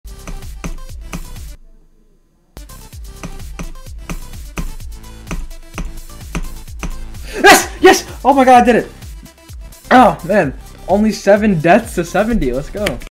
8 Jumps of Hell 100%
I actually really underrated this, took me around 3000att and 7 deaths to 70 but fluked the last jump so kinda balanced out.